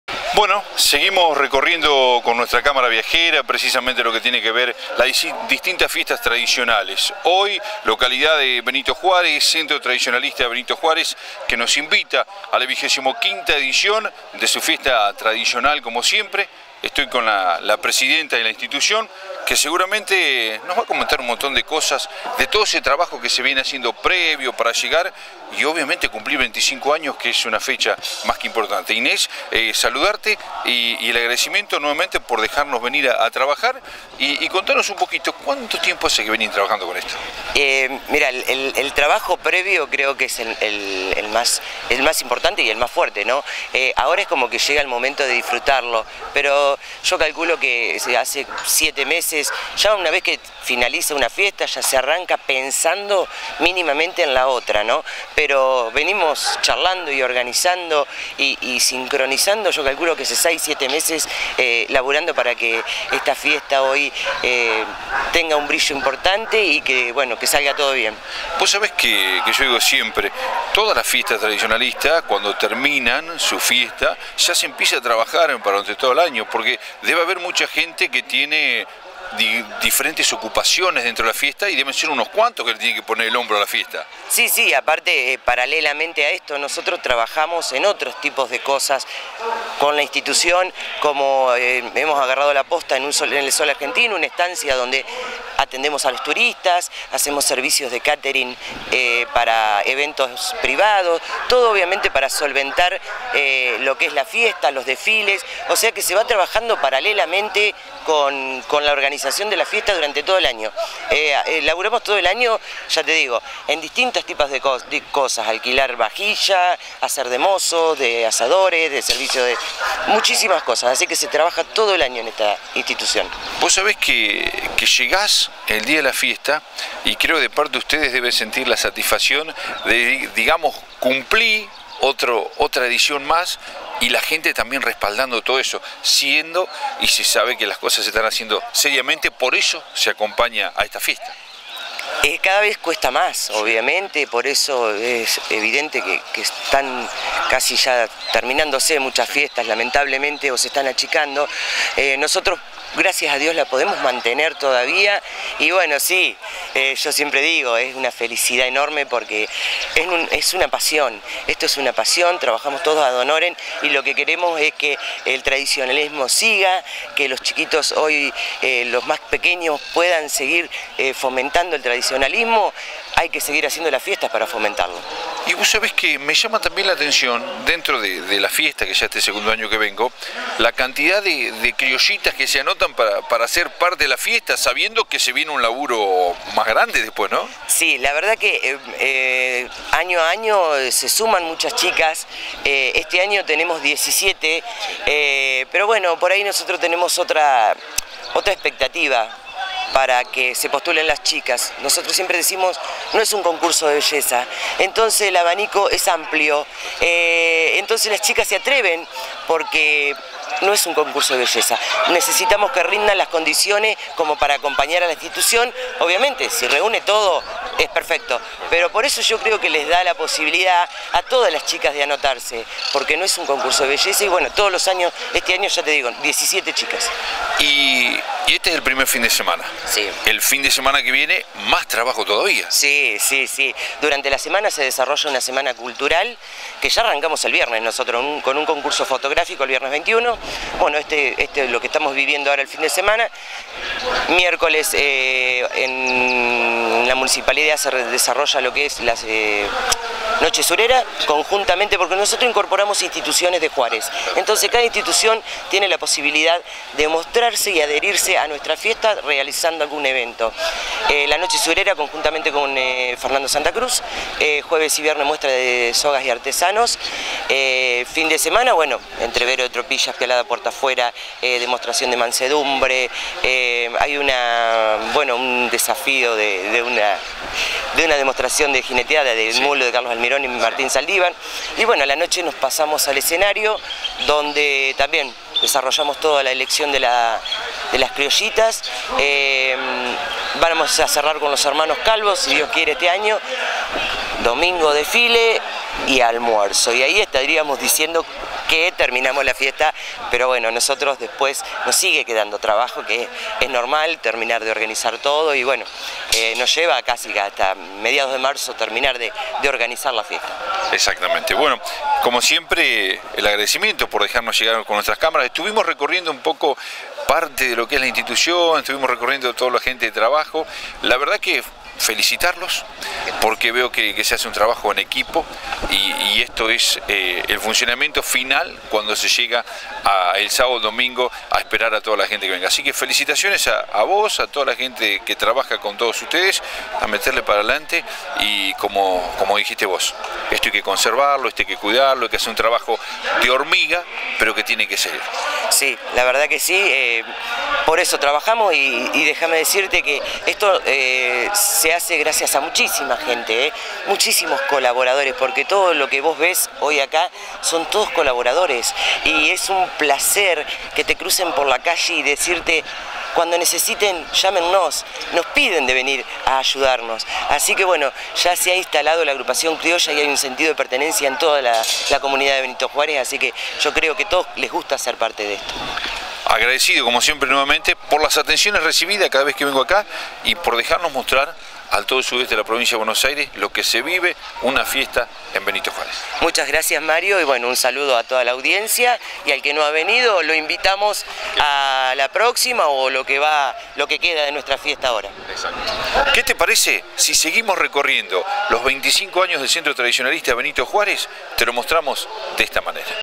UNA COPLA EN LA LLANURA - Nota Ines Salomon
Fiesta Agrupación Criolla Benito Juarez 2020 - nota realizada a la presidenta de la institución